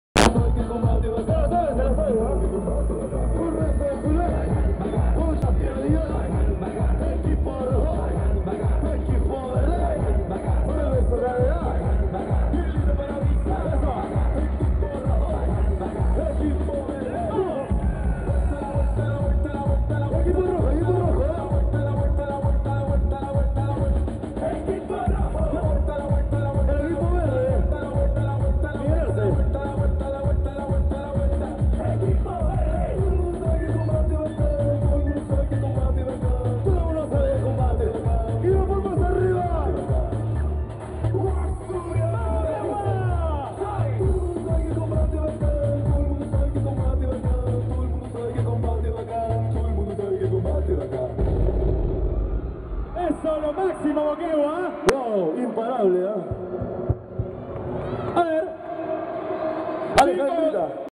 MOQUEGUA - COMBATE ES BACAN 20/10/12
Sábado 20 de Octubre desde 4: 00 p.m. Combate ATV en el Coliseo Municipal de Moquegua. más información

Paloma Fiuza, Fabianne hayashida, la "China", Miguel Arce, Jenko del Rio, Mario Irivarren, Alejandra Baigorria, Oscar Alejandro Benites "Zumba",Carlos La Pantera Zegarra.